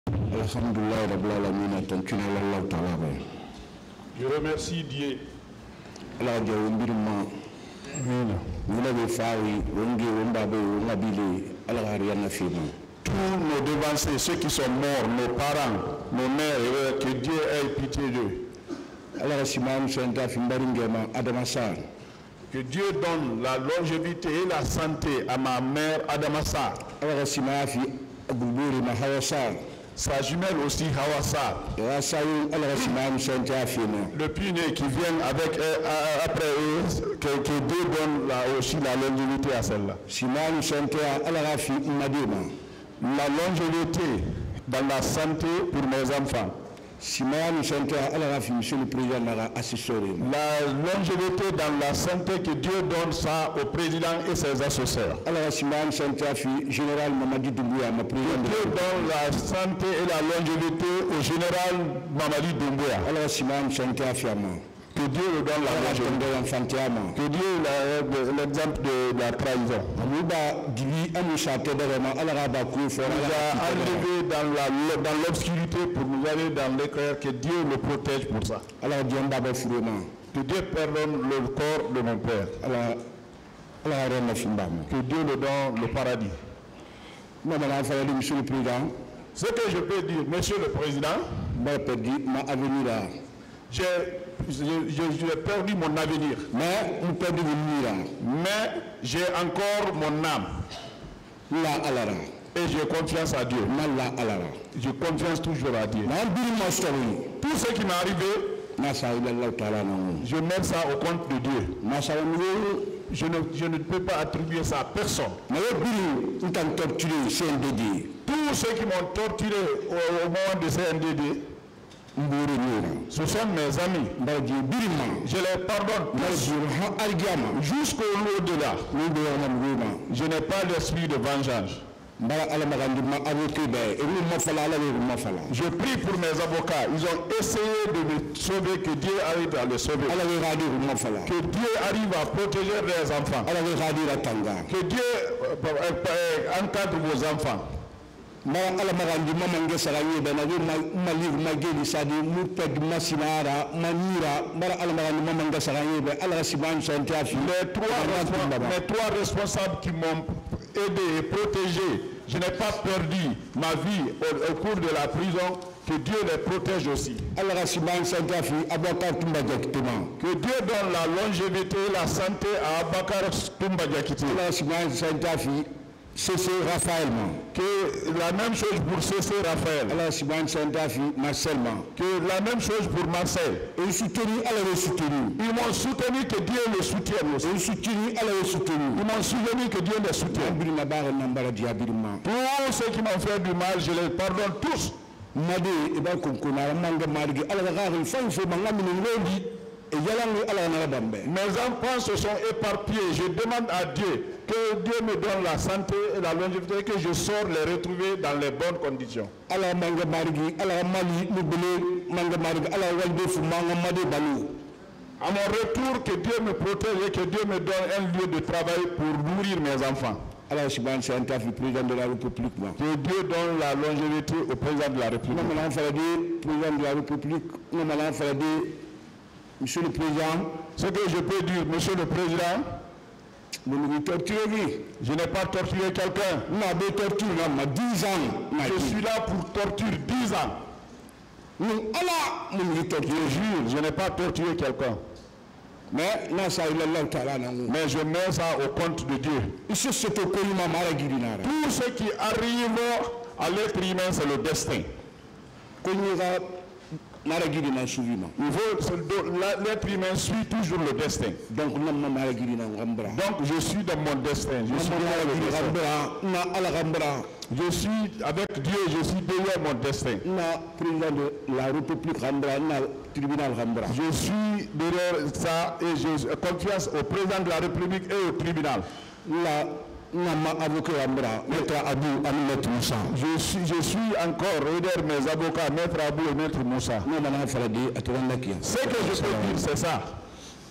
🔴Procès du 28 septembre « J’ai perdu mon avenir, mais j’ai encore mon âme et j’ai…» Mansa Guilavogui
📺 Actualité du 27 juin 2024
🎥 intégral